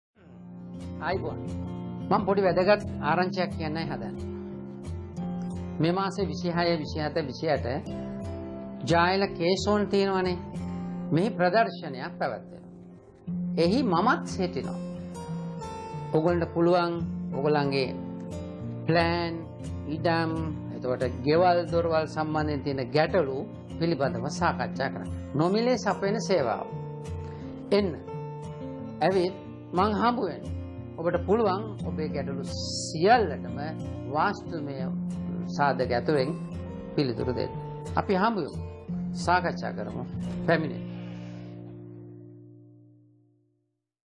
මෙම මස 26, 27, 28 අපි එනවා ජා ඇල K-Zone එකට..
මෙම මස 26, 27, 28 අපි එනවා ජා ඇල K-Zone එකට.. ඔබට වාස්තු සම්බන්ධව ඇති සියලු ගැටලු සාකච්ජා කිරීමට එදාට එන්න.. ඔබට මා හමුවී සහ අපේ කාර්ය මණ්ඩලය හමුවී ඔබගේ නිවසේ, ඉඩමේ ව්‍යාපාරයේ ඇති සියලුම ගැටලු මෙහිදී නොමිලේ නිරාකරණය කර ගැනීමට අවස්ථාව ලැබෙනවා.. නොවරදවාම එන්න..